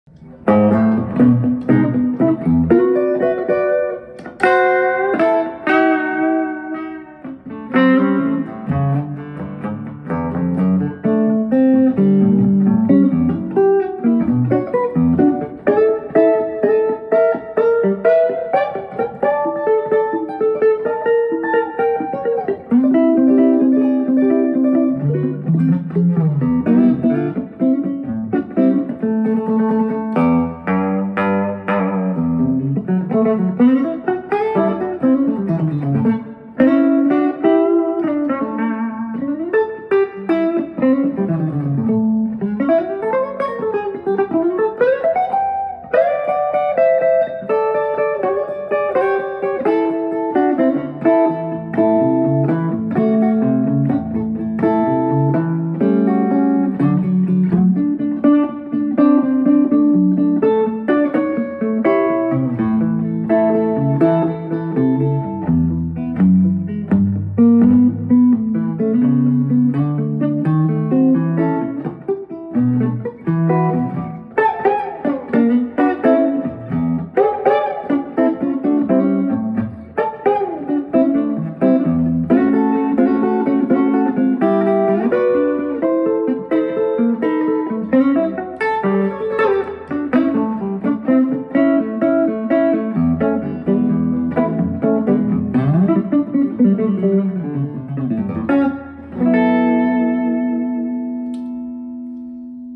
Jules Belmont and the Grez Grand Tour
Jules Bemont playing Grand Tour through a Henriksen Bud Six.

The Grand Tour is a 15" carved top archtop with a 2" body depth at the rim and a 16th fret neck attachment weighing just 5.8 lbs.  Featuring our unique Sprung Mass Tuning, the lowest body resonances are preserved creating an rich and larger acoustic sound.

Produced in 8 weeks with limited customer customization available.  Color, pickups, tailpiece, some wood and neck dim options too.